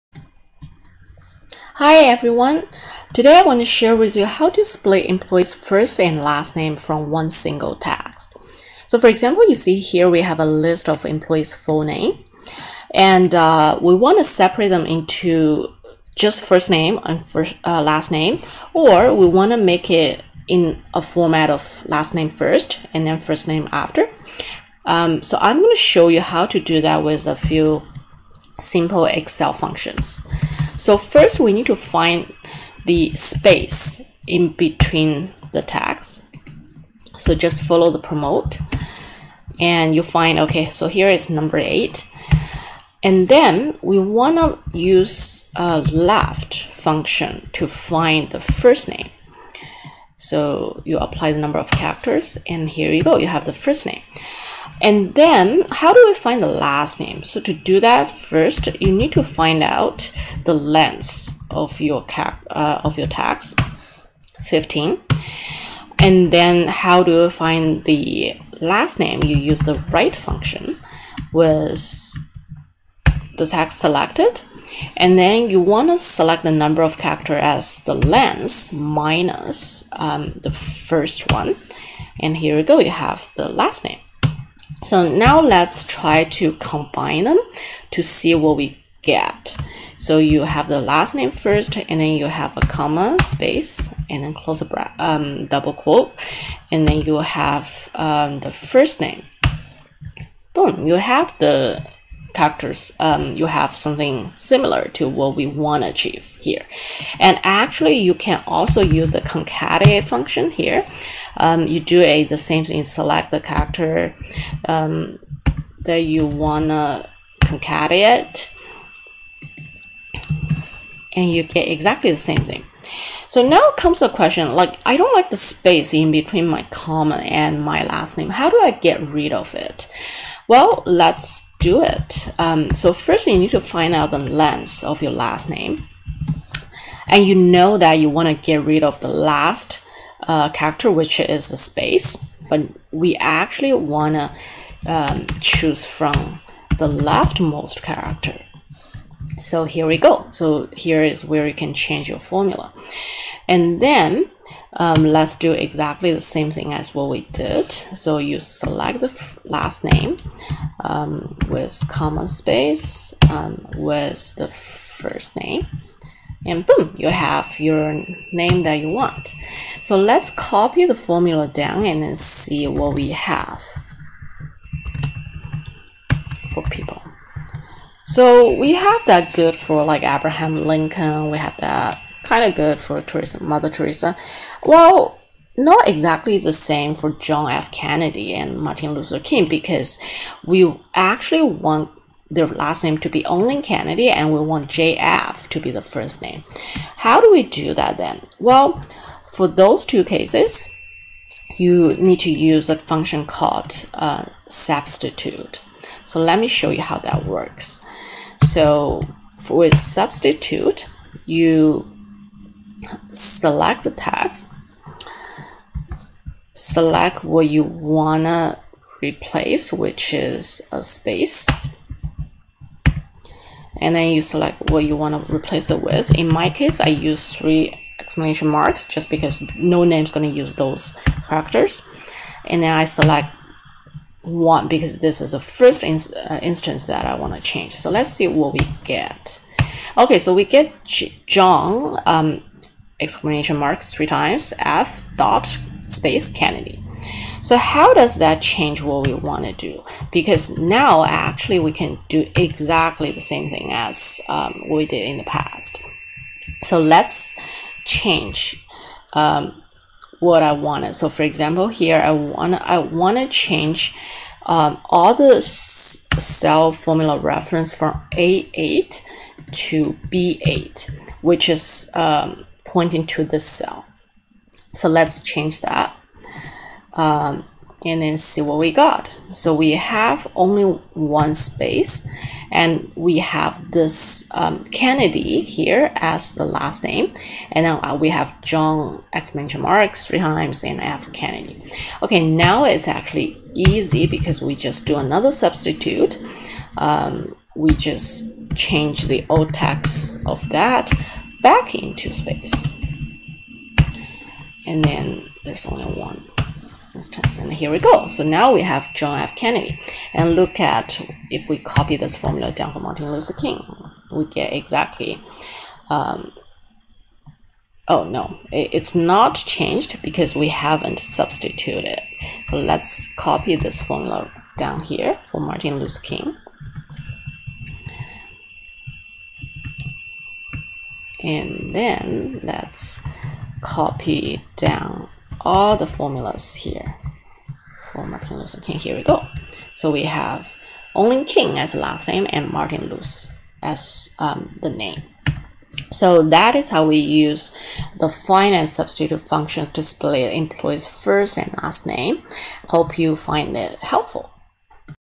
Excel for HR: Find and Substitute to split Employee First and Last Names from 1 single text
Business Scenario: How to split Employee First and Last Names from 1 single text
Excel Function: Find(), Right(), Left(), Len(), Substitute()
Additional case: What if your employee name has a middle name?